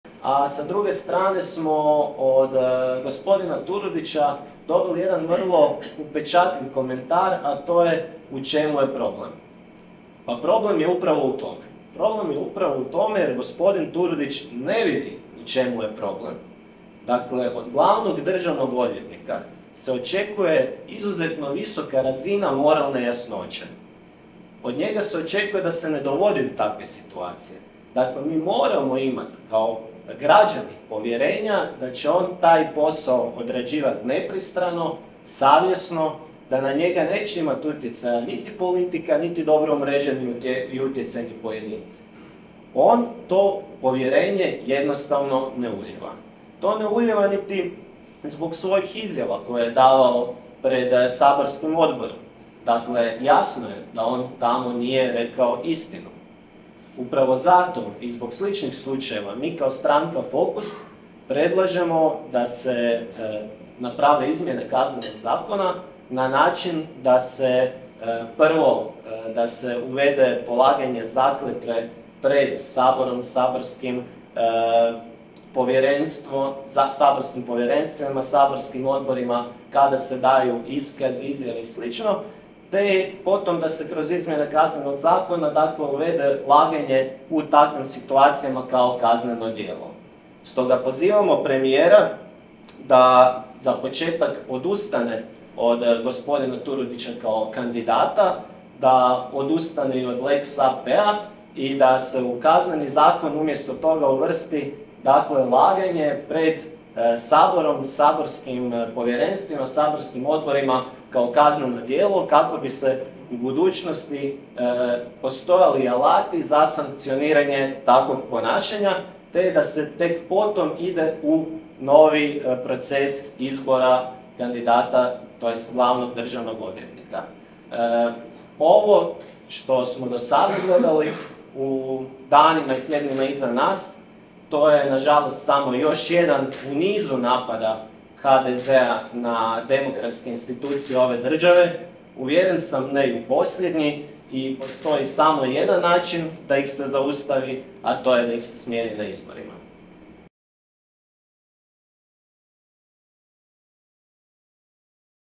Saborski zastupnik Davor Nađi o Ivanu Turudiću
Zagreb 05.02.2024 - Saborski zastupnik Davor Nađi o Ivanu Turudiću. Video: Damjan Tadić / CROPIX